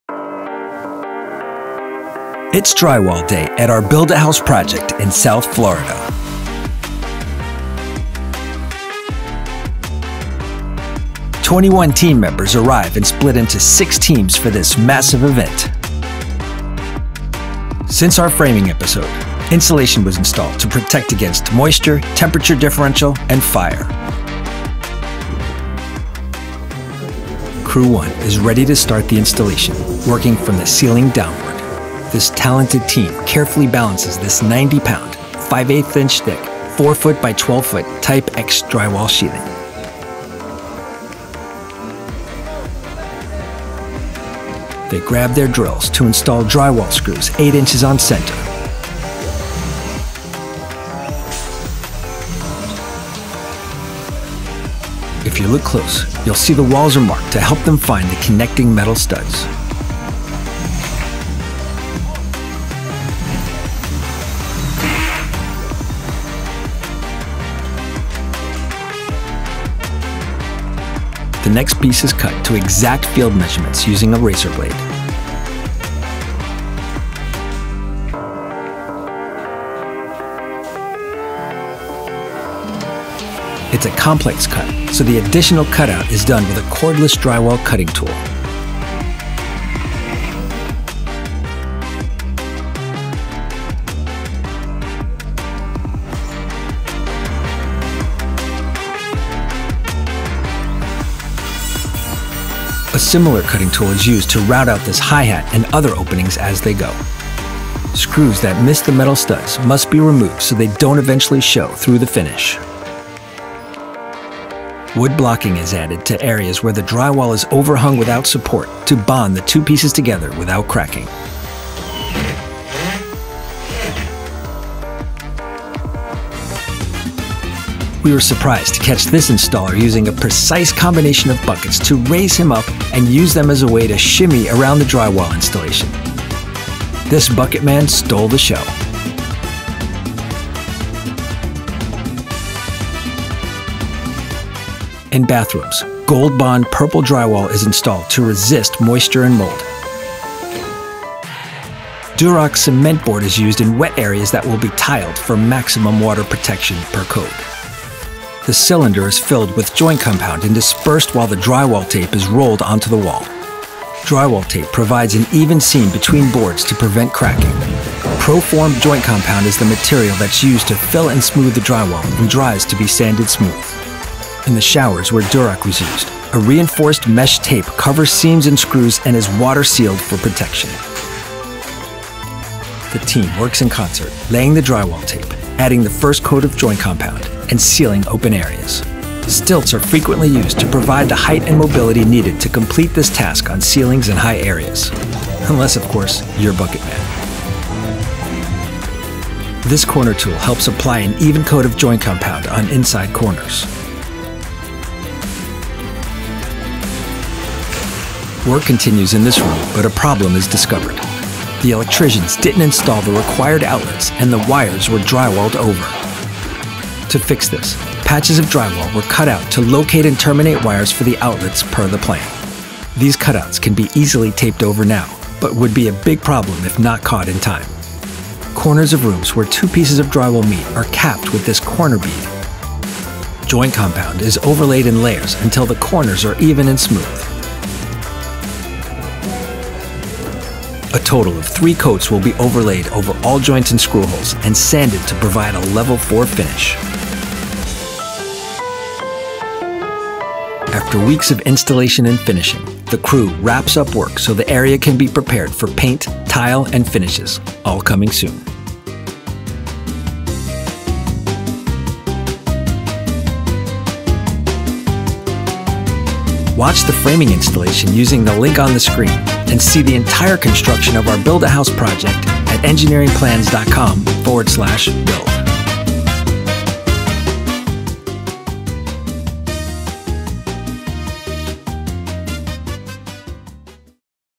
How to Install Drywall. Watch Cutting, Hanging, Taping, Applying Compound, and Sanding in a New Home
Want to Learn How to Install Drywall?

Watch the installation of drywall at this two-story house in South Florida!

See close-ups of drywall cutting, drywall hanging, taping, applying joint compound, and sanding in this engaging micro-documentary. You will learn so much by watching the pros do it!

*** Watch the AMAZING 'Bucket Man' fastest drywaller on earth!***

( Includes an amazing sequence where  one installer stacks paint buckets and shimmies around the home to get his drywall work done more quickly. )

This how-to video explains the overall process to those not familiar with the steps of drywall installation.

Watch 21 team members in 6 drywalling teams. We start from the ceiling downwards using 5/8 inch think 4 x 12 foot type X drywall sheathing.  They use their drills to install drywall screws. The walls are marked to help them locate the metal studs.

Watch the team cut the drywall to exacting measurements including more complex cuts using cordless drywall cutting tools.

Wood blocking is added to areas where the drywall is hung without support to bond the 2 pieces together without cracking.

Watch one installer using a precise combination of stacked buckets to shimmy around the home!

Gold Bond purple drywall is installed to resist moisture.

Durock cement board is used in wet areas that will be tiled. See the proper application of drywall tape. Watch as they add Pro Form joint compound to seal certain areas. See the use of corner tools to apply the right amount of compound

See how we overcome an issue where the electricians didn't install the required outlets and the wires were drywalled over.

Learn more about Engineering Plans and our Build a House Series!

Watch the entire 'Build a House' series